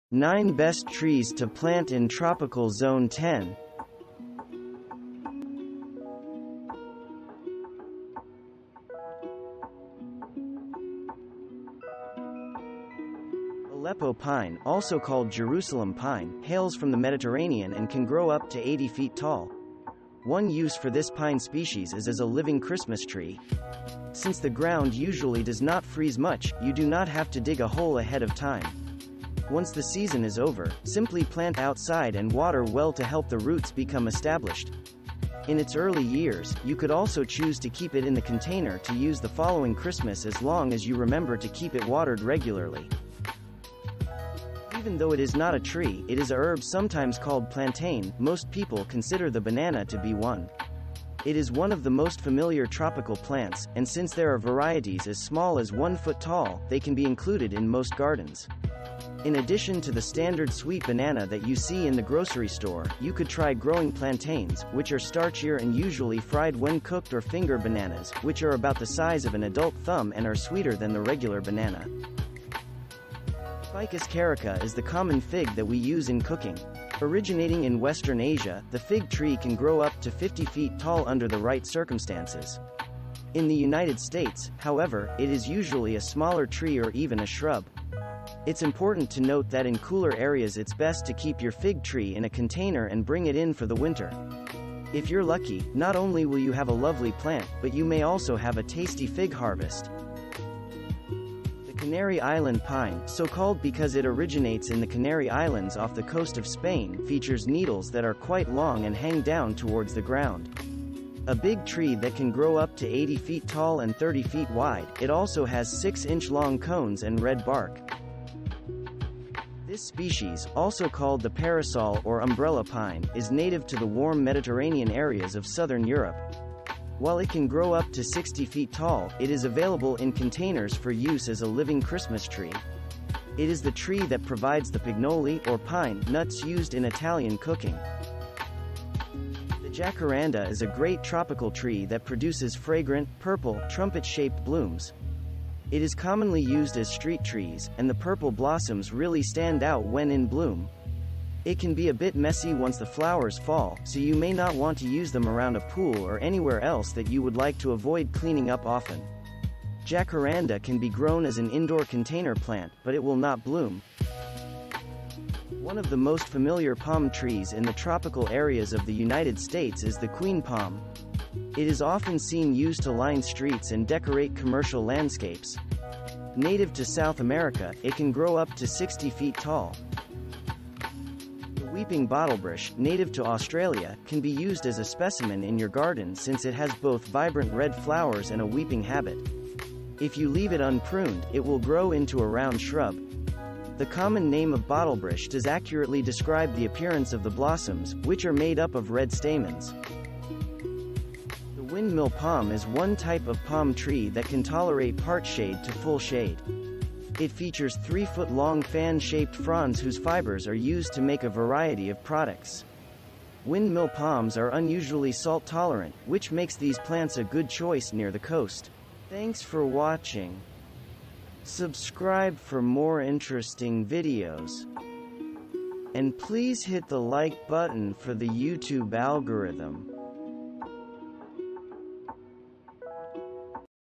9 Best Trees To Plant In Tropical Zone 10 🧧
Zone 10 is one of the warmest zones found in the United States. The trees that grow in this tropical environment have many colorful offerings.
0:00 - Intro
0:00:12 -  Aleppo Pine (Pinus halepensis)
0:00:21 -  Banana Tree (Musa spp)
0:00:30 -  Common Fig (Ficus carica)
0:00:39 -  Canary Island Pine (Pinus canariensis)
0:00:48 -  Italian Stone Pine (Pinus pinea)
0:00:57 -  Jacaranda (Jacaranda mimosifolia)
0:01:06 -  Queen Palm (Syagrus romanzoffiana)
0:01:15 -  Weeping Bottlebrush (Callistemon viminalis)
0:01:24 -  Windmill Palm (Trachycarpus fortunei)